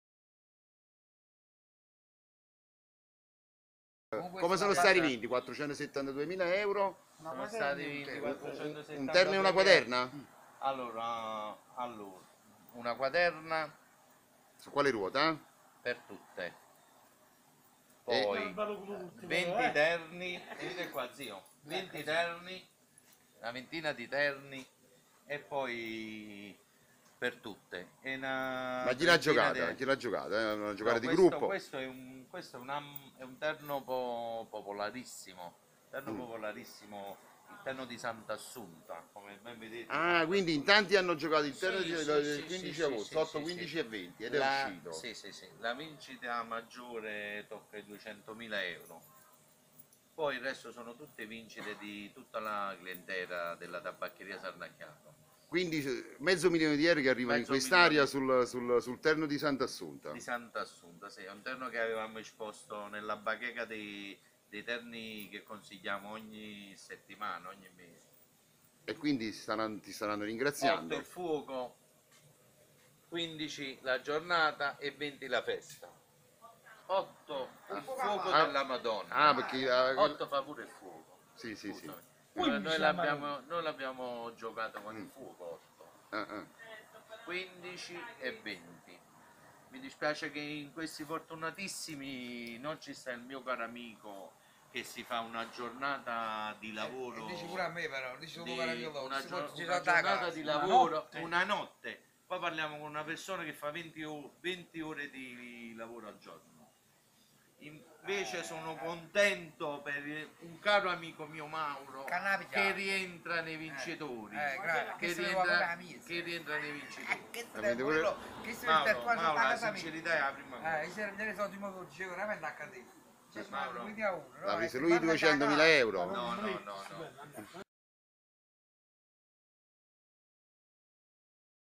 Mezzo milione di euro a Cardito con il terno di Santa Assunta
La proposta di Sandro Sarnacchiaro accolta dai suoi clienti ha portato fortuna